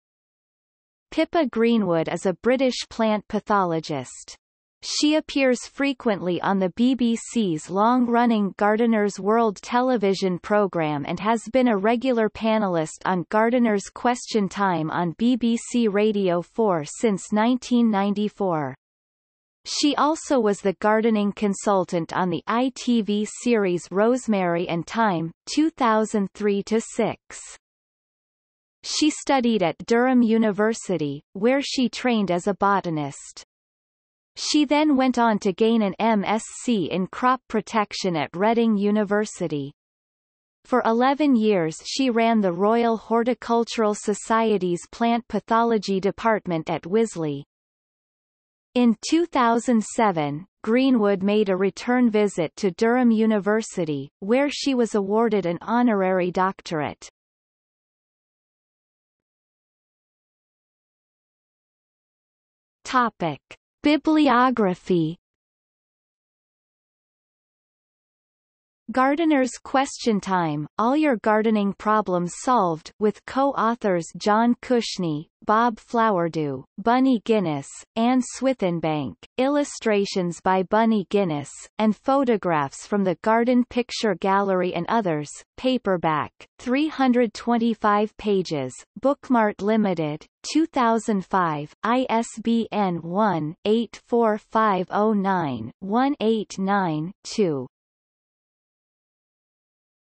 Pippa Greenwood | Wikipedia audio article
00:00:59 Bibliography

- Socrates

SUMMARY
Pippa Greenwood is a British plant pathologist. She appears frequently on the BBC's long running Gardeners' World television programme and has been a regular panelist on Gardeners' Question Time on BBC Radio 4 since 1994. She also was the gardening consultant on the ITV series Rosemary and Thyme (2003–6)
She studied at Durham University, where she trained as a botanist. She then went on to gain an MSc in crop protection at Reading University. For eleven years she ran the Royal Horticultural Society's Plant Pathology Department at Wisley.
In 2007, Greenwood made a return visit to Durham University, where she was awarded an honorary doctorate.